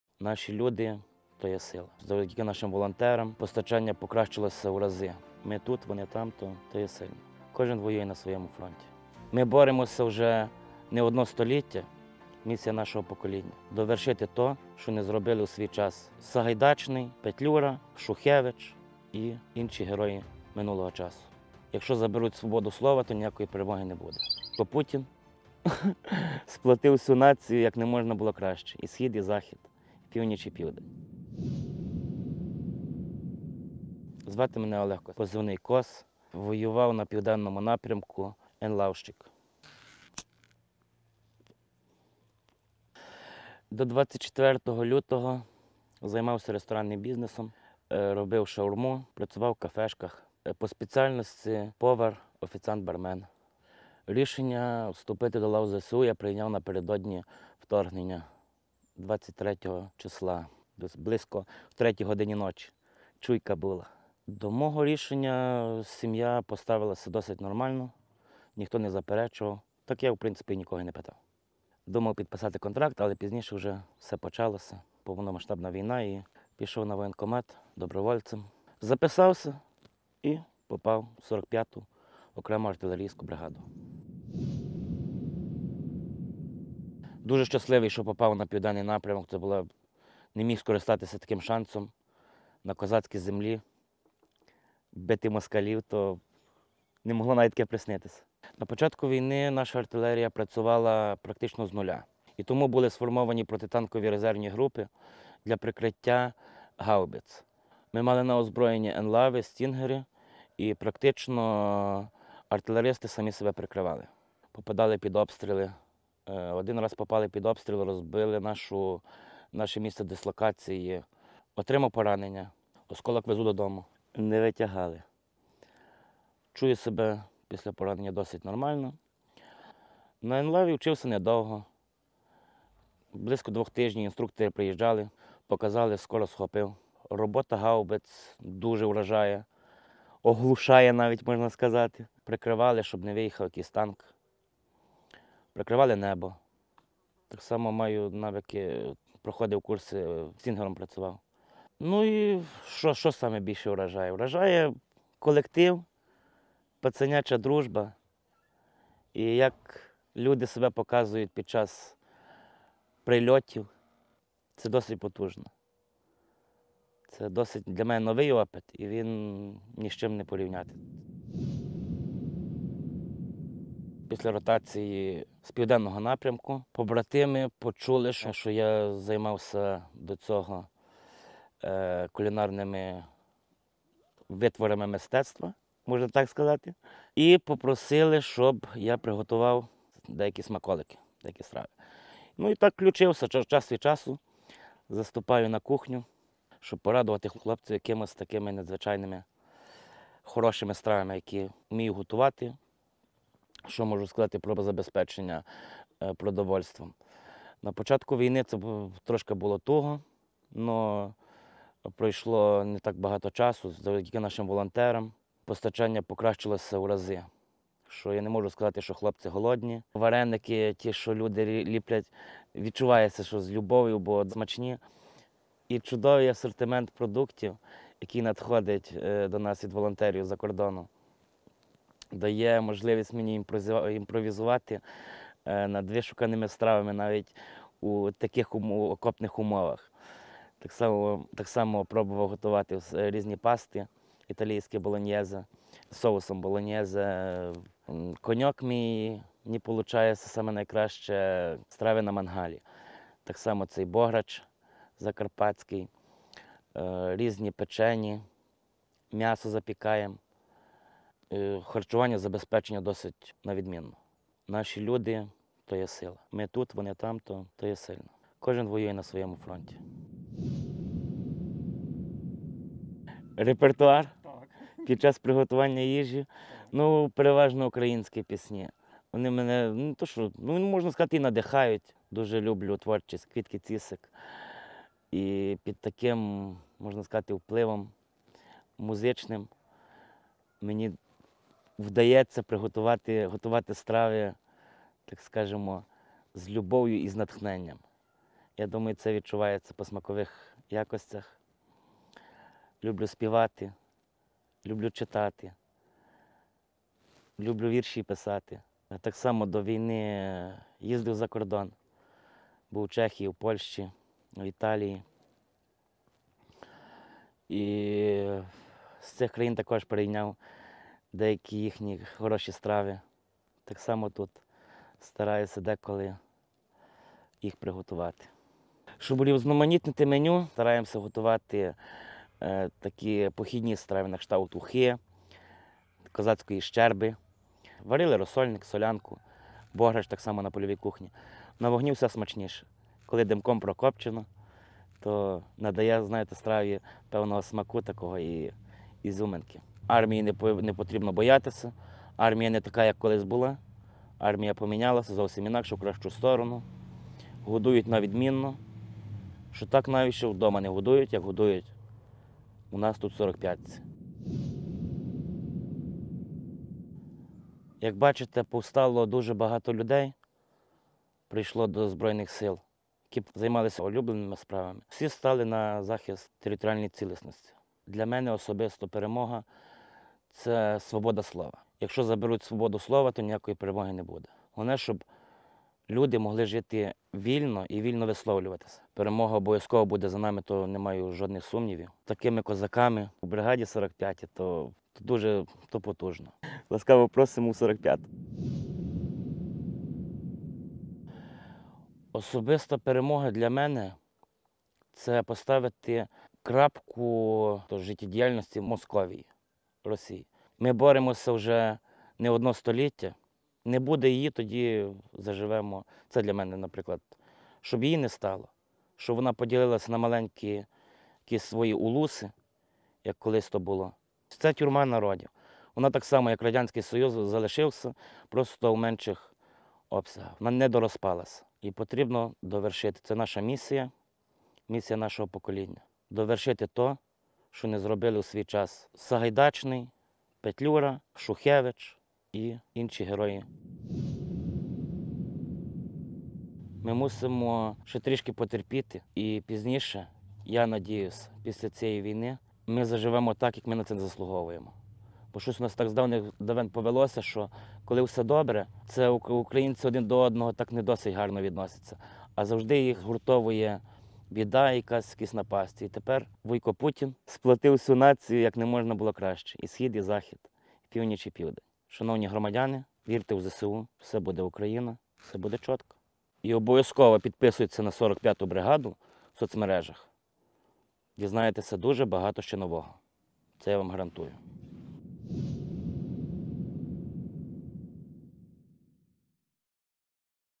45 оабр: «Ми повинні завершити те, що не зробили у свій час Сагайдачний, Петлюра, Шухевич»!
Після повномасштабного вторгнення і загальної мобілізації до лав ЗСУ прийшла велика кількість різноманітних спеціалістів. Кожен із них має свій власний життєвий досвід, професійні навички, власні таланти. Ця різноманітність згуртовує нас і дає поштовх для розвитку Збройних сил, зміцнення військових знань і досвіду. У цей же час наші тили забезпечують самовіддані, енергійні та ефективні різноманітні волонтерські спільноти.
Позивний Кос: «Наші люди то є сила!»